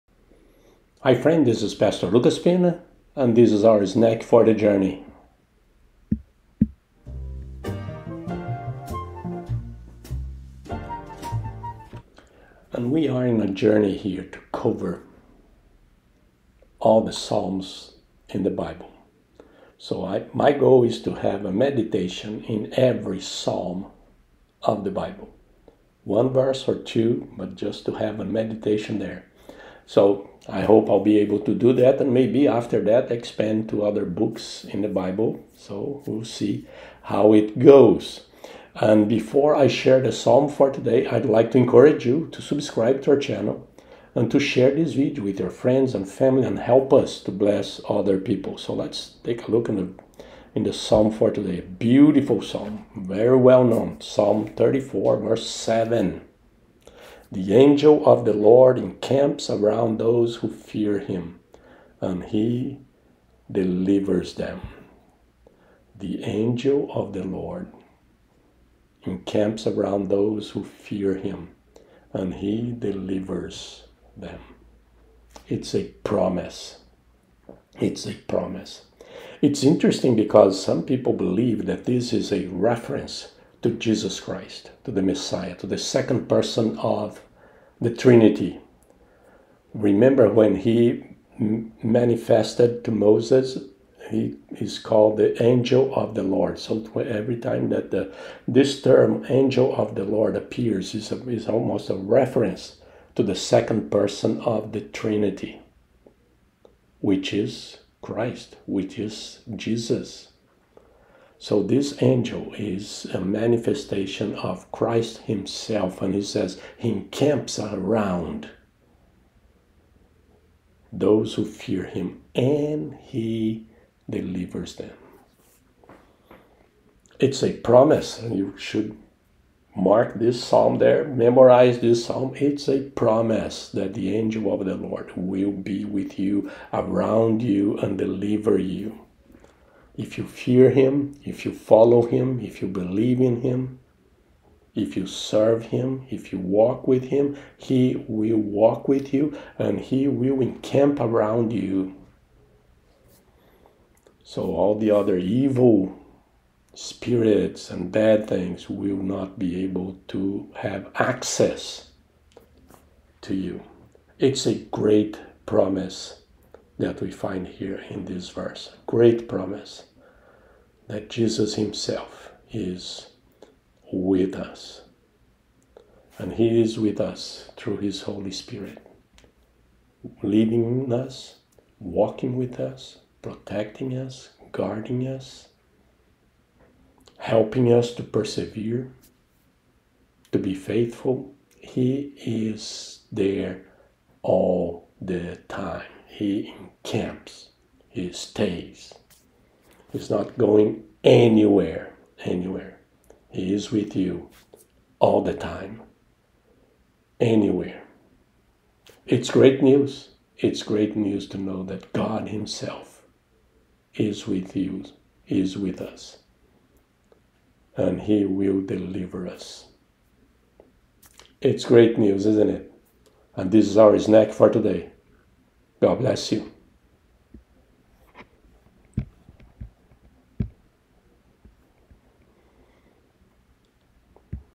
Snack for the Journey - #1246 - Psalm 34:7
This is just a short meditation based on Psalm 34:7.